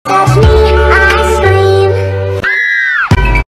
If CapCut added 3D... 🤯😳
QOTD: Do you think CapCut will have a 3D setting in the future?

This isnt real but IMAGINE OMG!

Sub Count 🤍💗: 150k Piinks 🌸 *ty all sm :D*